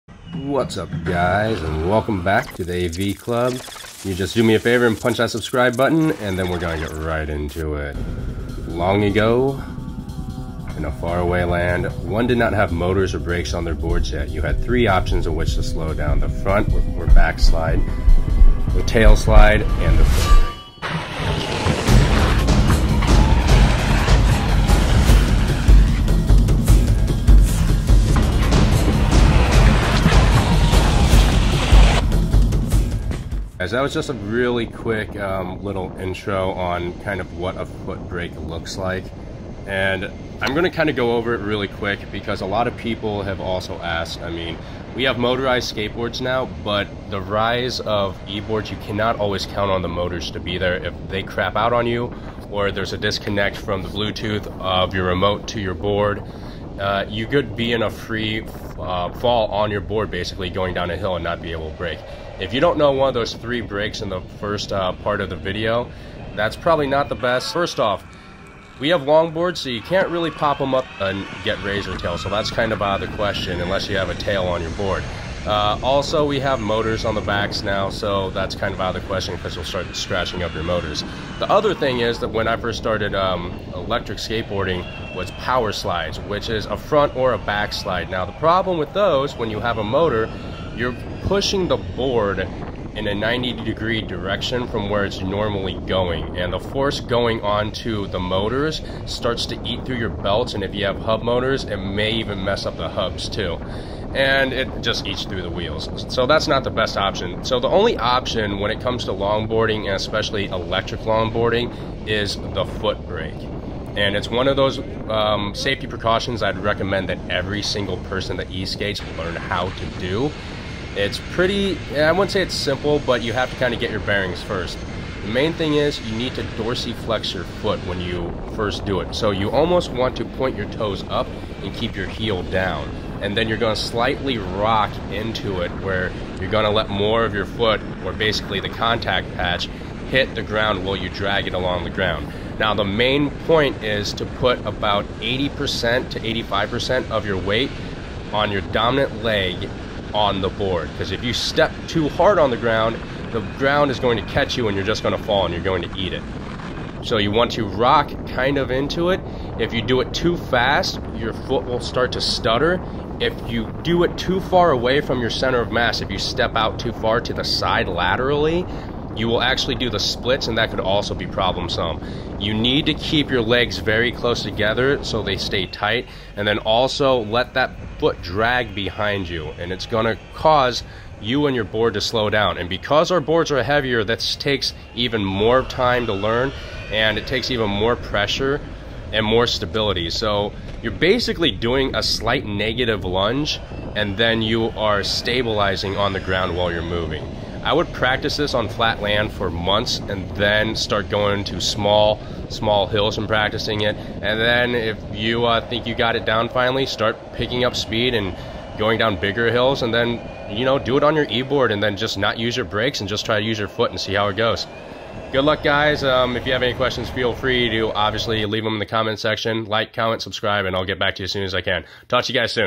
THE LONGBOARD FOOTBRAKE | EPISODE#154
What's up Esk8 fam?

Today I decided to go over an old tactic to slowing down your board manually, the footbrake.

It is a great move to slow yourself down, when going down a hill or at speed. NOTE...It takes time to learn. Days, weeks...maybe months.

Tricks-
-Figure out which leg is dominant and use that one to stay on the board.
-When you foot brake, keep the foot close as possible to the board
-Dorsilfex the foot and roll it down slowly
-Start to drag and add more pressure slowly to slow down more

NOTE: IF YOU DO THIS AT HIGH SPEEDS YOUR FOOT WILL ACTUALLY BE ABOUT 3-6 INCHES BEHIND YOU BECAUSE OF THE INSANE FORCE BEING CAUSED.

NOTE: DO NOT STEP OUT LATERALLY OR YOU WILL DO THE SPLITS.

Shot on Hero8
Edited iMovie